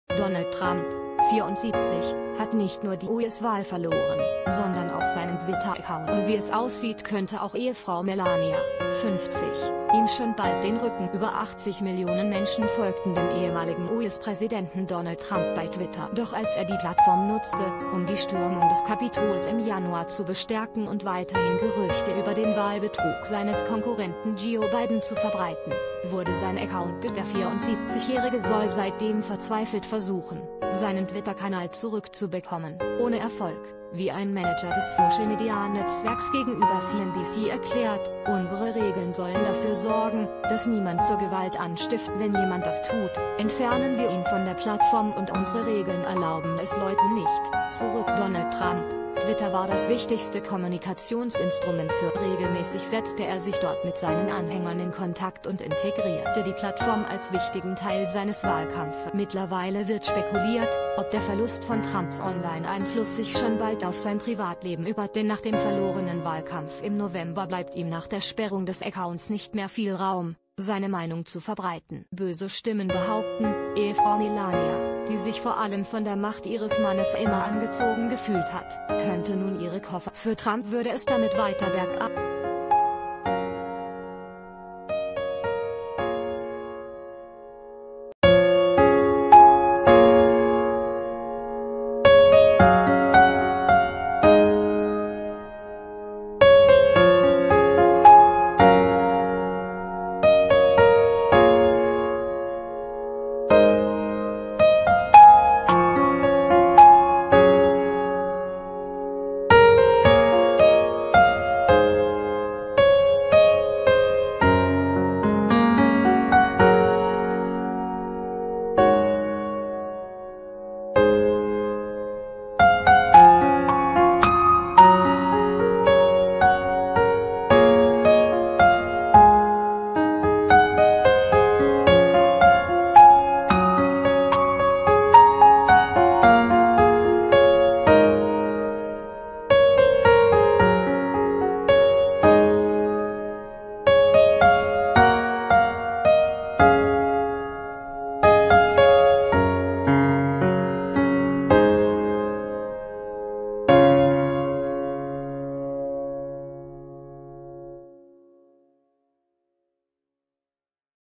Donald & Melania Trump: Herber Schlag!
Donald Trump, 74, hat nicht nur die US-Wahl verloren, sondern auch seinen Twitter-Account. Und wie es aussieht könnte auch Ehefrau Melania, 50, ihm schon bald den Rücken kehren.   Über 80 Millionen Menschen folgten dem ehemaligen US-Präsidenten Donald Trump bei Twitter. Doch als er die Plattform nutzte, um die Stürmung des Kapitols im Januar zu bestärken und weiterhin Gerüchte über den Wahlbetrug seines Konkurrenten Joe Biden zu verbreiten, wurde sein Account gesperrt.  Der 74-Jährige soll seitdem verzweifelt versuchen, seinen Twitter-Kanal zurückzubekommen - ohne Erfolg, wie ein Manager des Social-Media-Netzwerks gegenüber CNBC erklärt: "Unsere Regeln sollen dafür sorgen, dass niemand zur Gewalt anstiftet. Wenn jemand das tut, entfernen wir ihn von der Plattform und unsere Regeln erlauben es Leuten nicht, zurückzukommen."        Donald Trump:   Twitter war das wichtigste Kommunikationsinstrument für Trump. Regelmäßig setzte er sich dort mit seinen Anhängern in Kontakt und integrierte die Plattform als wichtigen Teil seines Wahlkampfes.  Mittlerweile wird spekuliert, ob der Verlust von Trumps online-Einfluss sich schon bald auf sein Privatleben übertragen könnte. Denn nach dem verlorenen Wahlkampf im November bleibt ihm nach der Sperrung des Accounts nicht mehr viel Raum, seine Meinung zu verbreiten.  Böse Stimmen behaupten, Ehefrau Melania, die sich vor allem von der Macht ihres Mannes immer angezogen gefühlt hat, könnte nun ihre Koffer packen. Für Trump würde es damit weiter bergab gehen ...